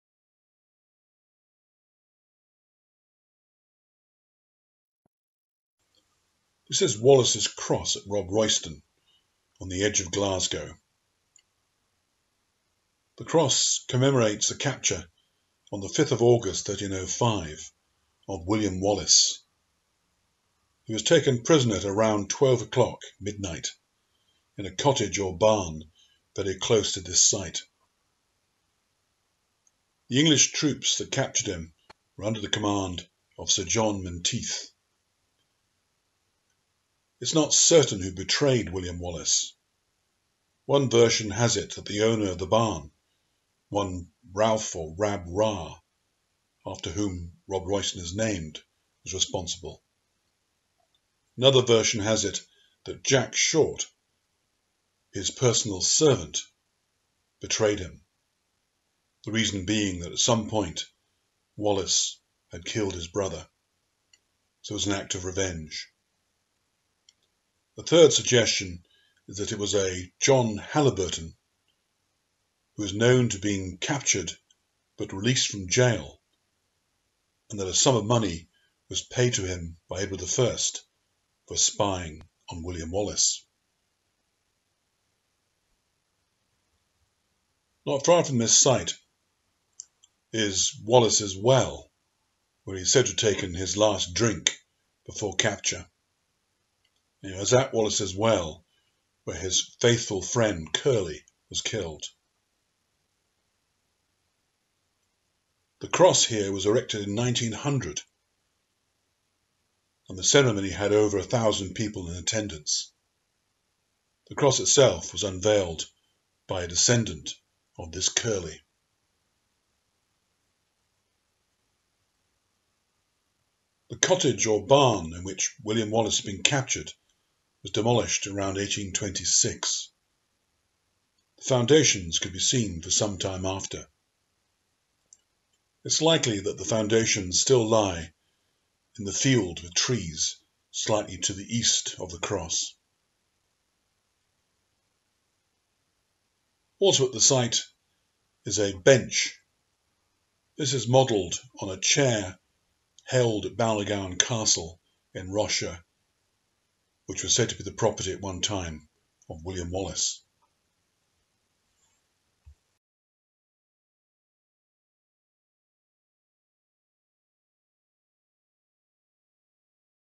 Wallace's Cross, Robroyston, Glasgow
The cross stands close to the site of the building in which William Wallace was captured on 5 August 1305. Wallace was betrayed by one of three people, Ralph Raa, Jack Short or John Haliburton and was taken prisoner by Sir John Menteith.